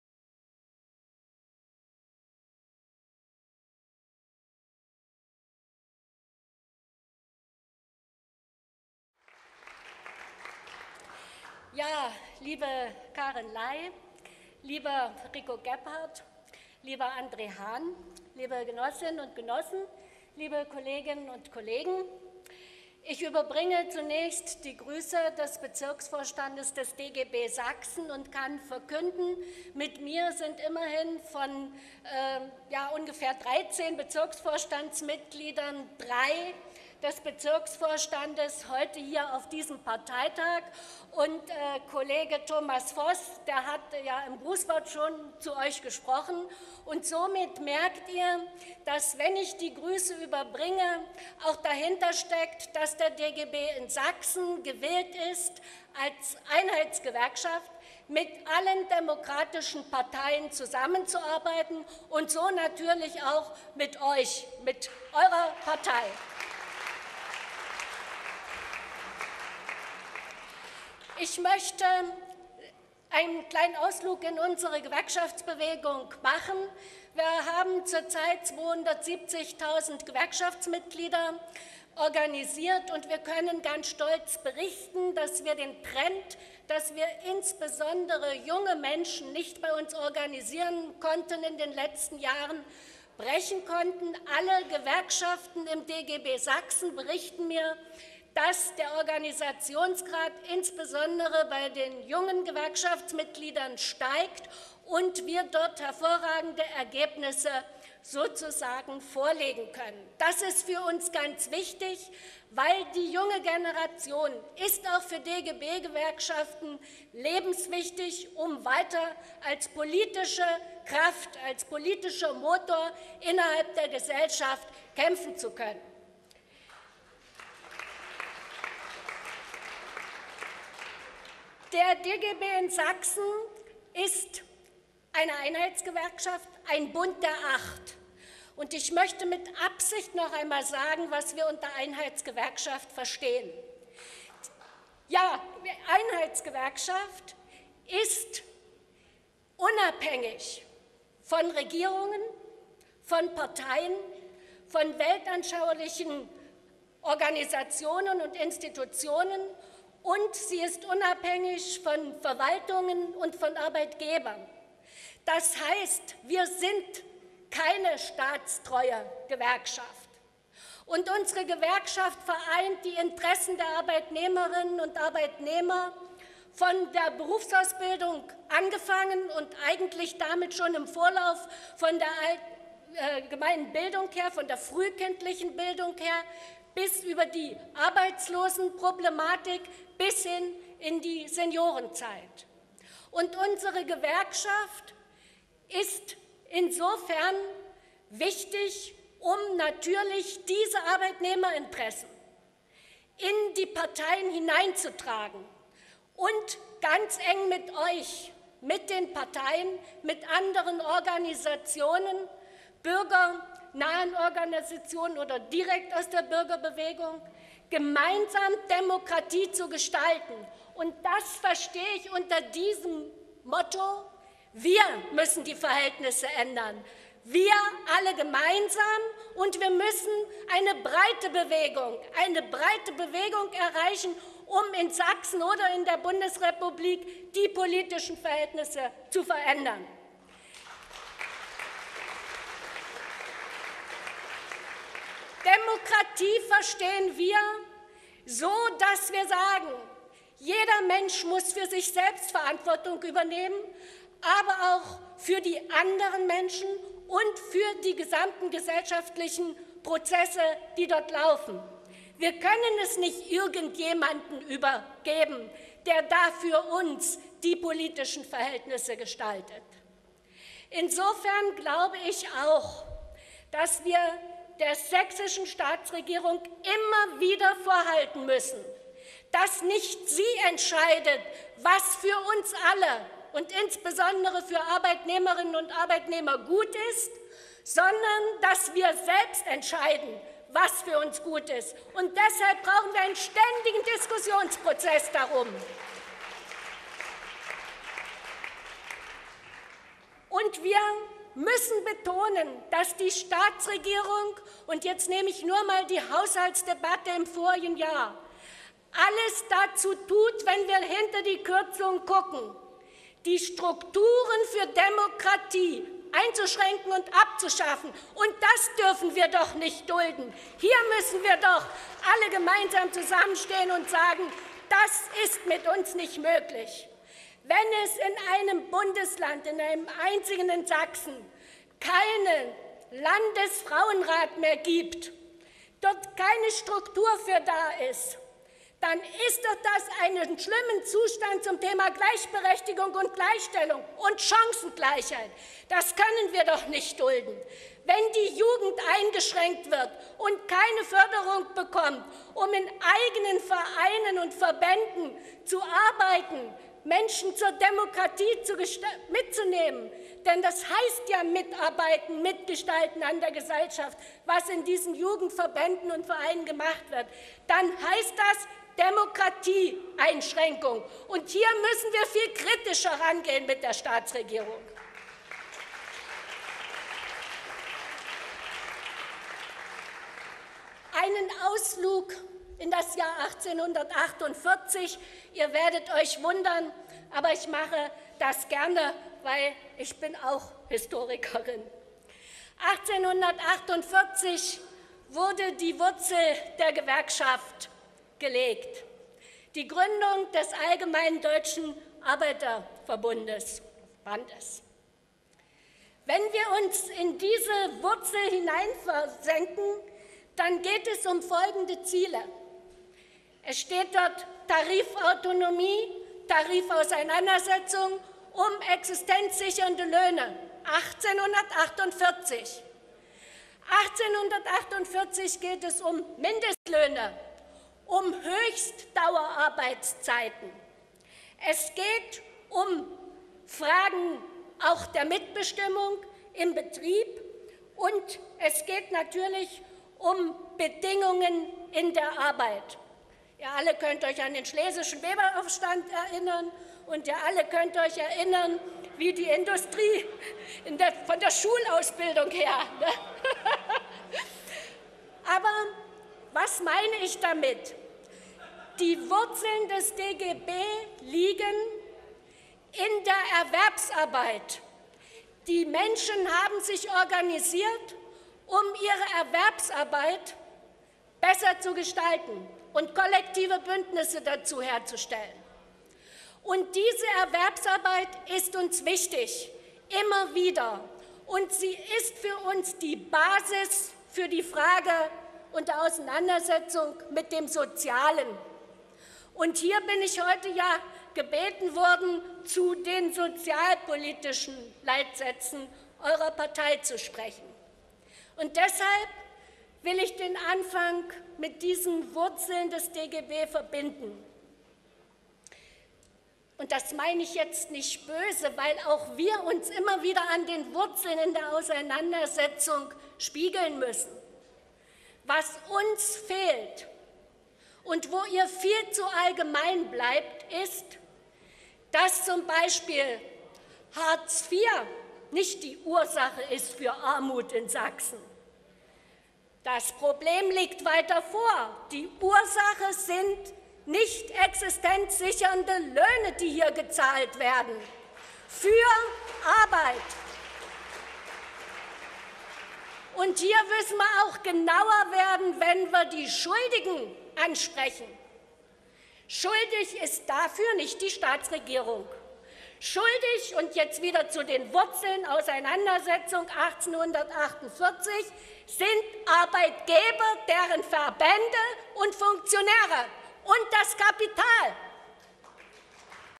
06. Redebeitrag Iris Kloppich (1v3)
DIE LINKE. Sachsen, 6. Landesparteitag, 5./6. November 2011 in Bautzen -  Redebeitrag von Iris Kloppich, Vorsitzende des DGB Sachsen zu den sozialpolitischen Vorstellungen der LINKEN Sachsen
aus Sicht der Gewerkschaften